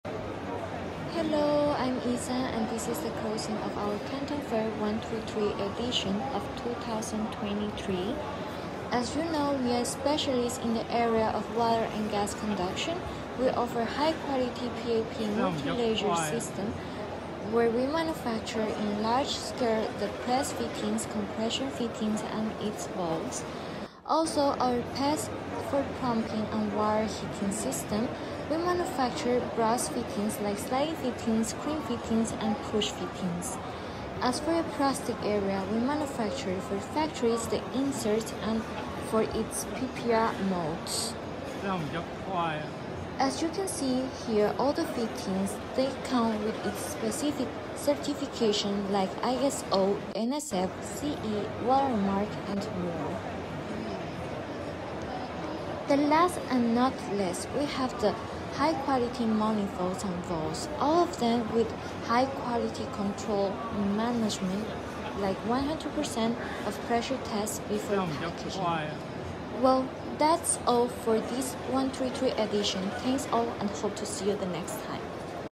The 133rd Canton Fair
The 133rd Canton Fair has just come to a close, marking yet another successful edition of one of the world's largest trade fairs.

Although the fair was held virtually for the third consecutive year due to the ongoing pandemic, We once again have the opportunity to connect in person with potential partners.

So to all of the participants that stop by our booth we want to say thank you for your time, and for those whom couldn´t make it this time, we can't wait to see you in person at the next event.